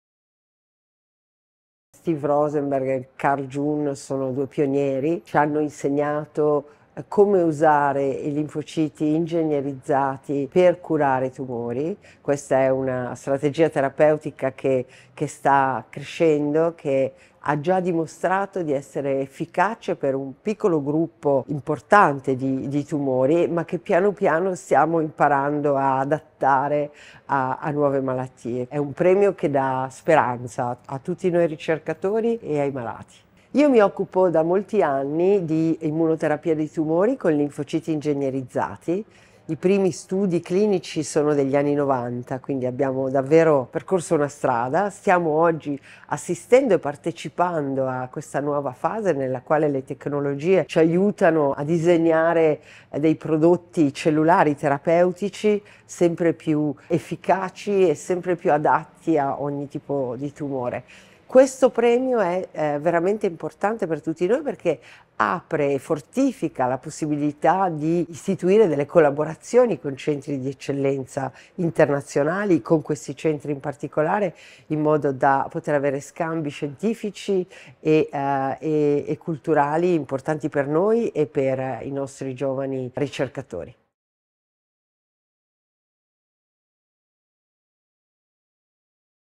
Chiara Bonini (San Raffaele): “Dal Premio di Regione una speranza per ricercatori e malati”
Sono molte le possibilità aperte, per i centri di ricerca lombardi impegnati in ricerche sull’immunoterapia, dall’assegnazione del Premio internazionale “Lombardia è Ricerca” 2023 di Regione Lombardia a due nomi di altissimo livello nel campo della ricerca contro i tumori.

Ce lo spiega la professoressa Chiara Bonini, Vice Direttore della Divisione di Ricerca di Immunologia, Trapianti e Malattie Infettive dell’IRCCS San Raffaele e Ordinario di Scienze Tecniche Mediche Applicate all’Università Vita-Salute del San Raffaele, a margine dell’ultima Giornata della Ricerca promossa da Regione Lombardia per la consegna del Premio Internazionale.